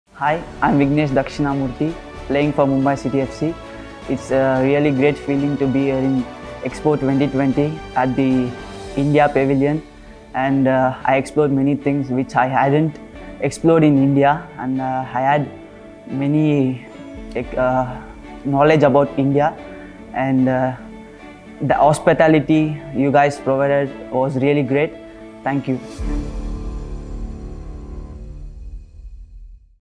Expo 2020 Dubai | India Pavilion | Mr. Vignesh Dakshinamurthy, Mumbai City FC
IndiaPavilion made me explore a lot about India. It is a great feeling to be here: Mr. Vignesh Dakshinamurthy, Indian Footballer & Defender for Mumbai City FC, at the pavilion at Expo2020Dubai.